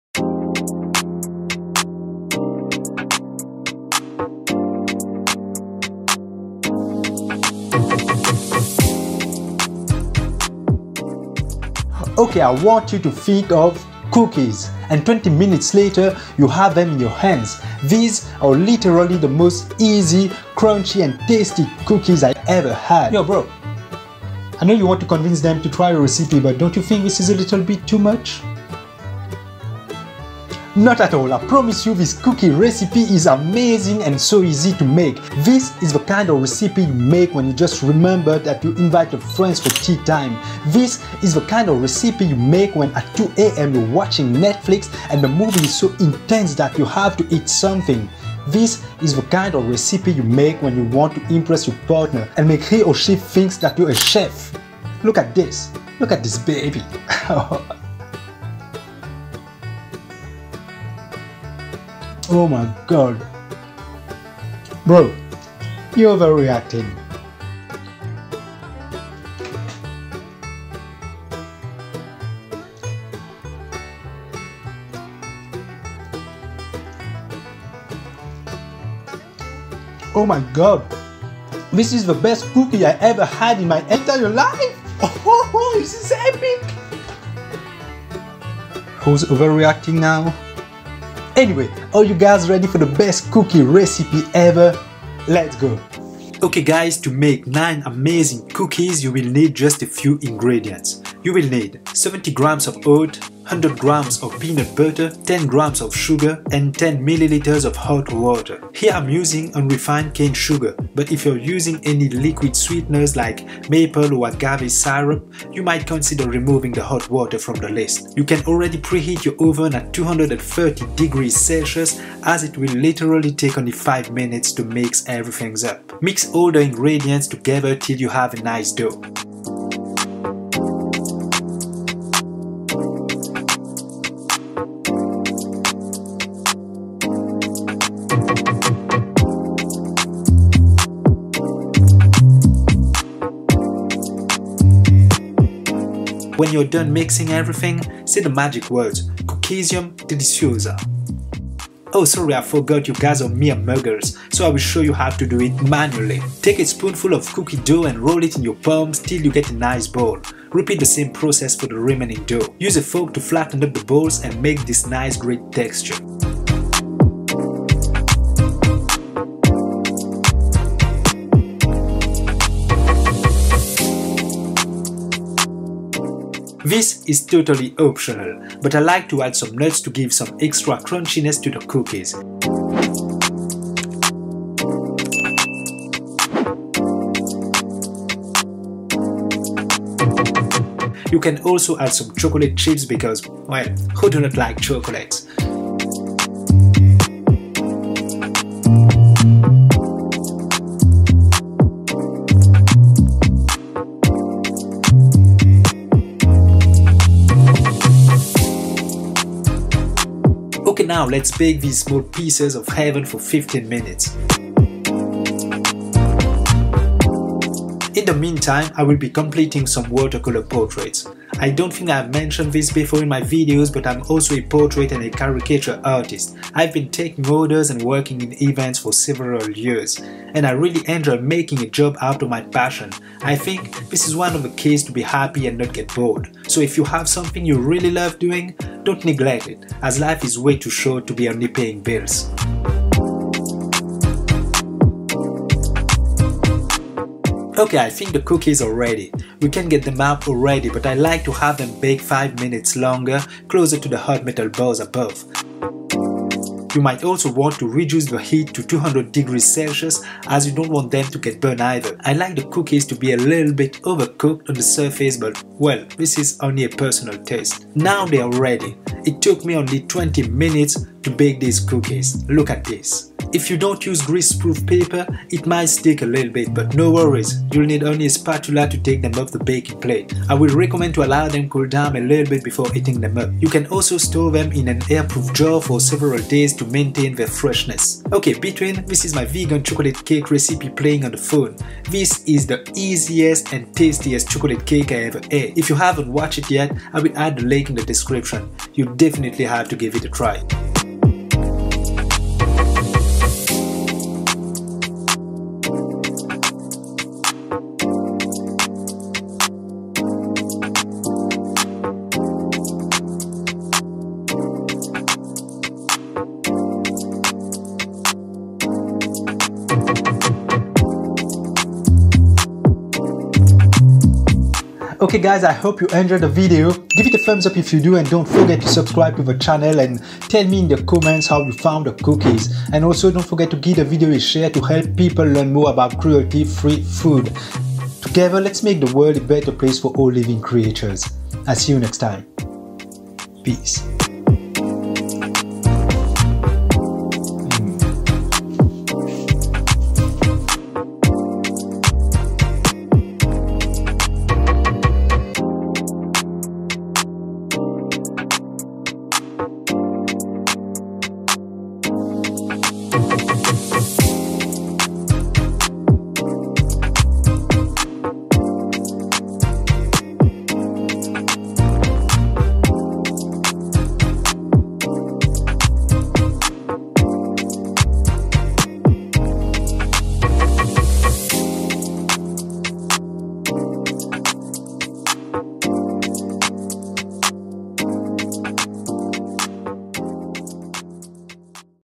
Lazy Cookies Recipe ( Eggless, Dairy free, Gluten Free ) Easiest and tastiest vegan cookies ever !!!
Well, the title says it all. This is the kind of recipe you make when you are just in the mood to eat and not bake :) You'll definitely have to invest some time in baking though but I promise you, it'll just be just a few minutes.
5 minutes for making the dough
15 more to bake.
In 20 minutes, you'll be holding in your hands these amazingly tasty and crunchy vegan cookies.

For 9 cookies / Ingredients list:
-70 g of Oat
-100 g of Peanut Butter
-10 g of sugar
-20 ml of hot water

Optional :
-A handful of hazelnuts or any nuts you have available
-Chocolate chips

Here's the link to the best and easiest chocolate cake you'll ever had. You'll thank me later :P

If you want to have a look at my art, here's my facebook page.

FILMING AND VIDEO EDITING
▶ Jonathan Naïgon